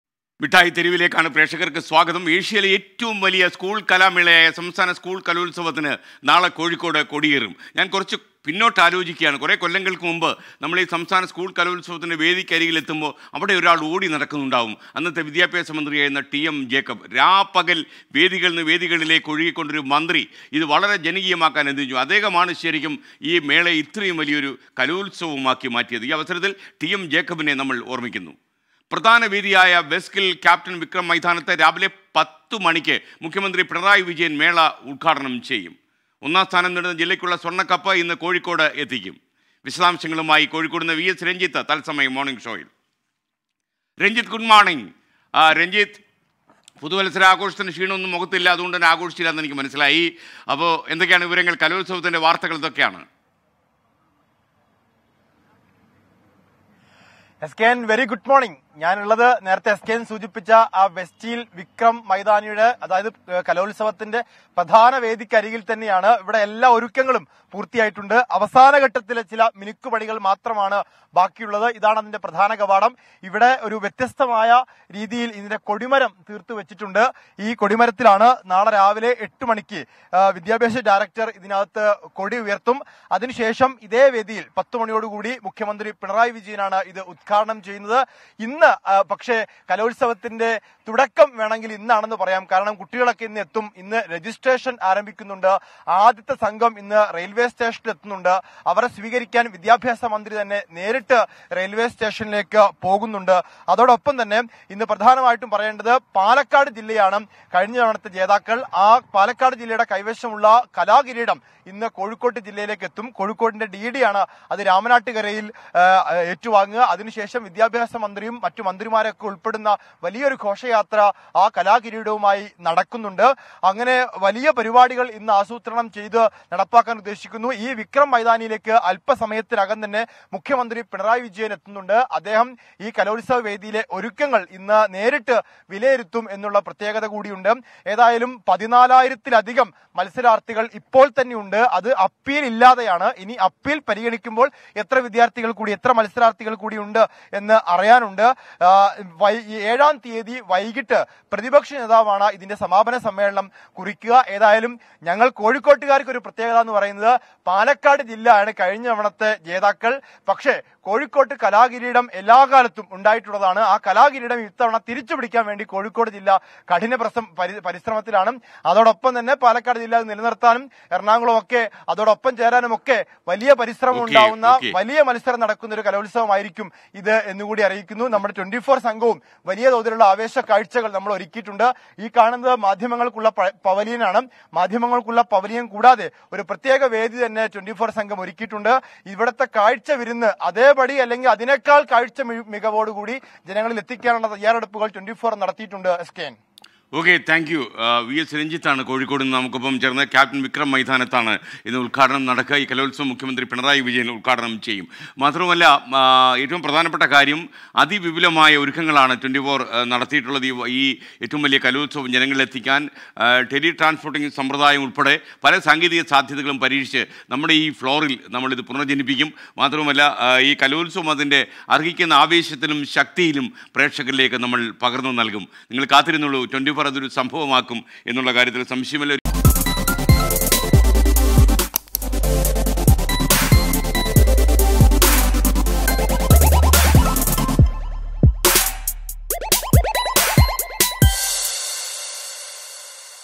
സംസ്ഥാന സ്കൂൾ കലോത്സവത്തിന് ഒരുങ്ങി കോഴിക്കോട്
സംസ്ഥാന സ്കൂൾ കലോത്സവത്തിന് ഒരുങ്ങി കോഴിക്കോട്; മുഖ്യമന്ത്രി പിണറായി വിജയൻ മേള ഉദ്ഘാടനം ചെയ്യും
Kozhikode ready for State School Arts Festival; Chief Minister Pinarayi Vijayan will inaugurate the fair